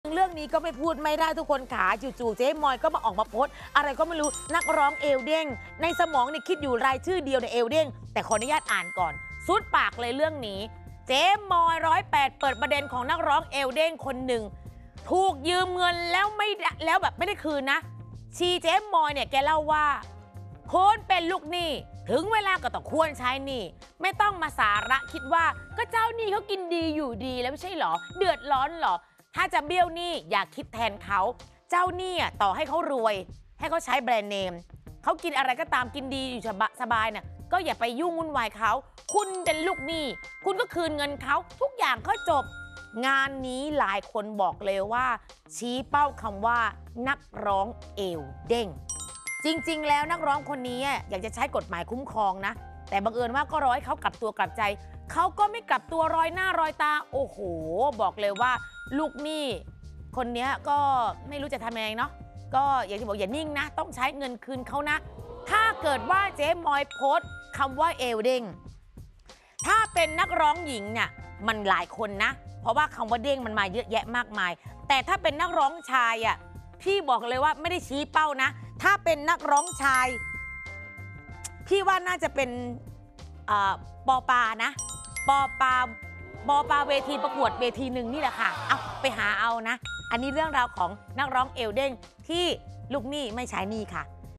แฉ! "ลูกหนี้" เบี้ยวคืนเงิน "นักร้องเอวเด้ง" ทำลอยหน้าเพราะได้กฎหมายคุ้มครอง I 12-04-66
แฉ! "ลูกหนี้" เบี้ยวคืนเงิน "นักร้องเอวเด้ง"  ทำลอยหน้าเพราะได้กฎหมายคุ้มครอง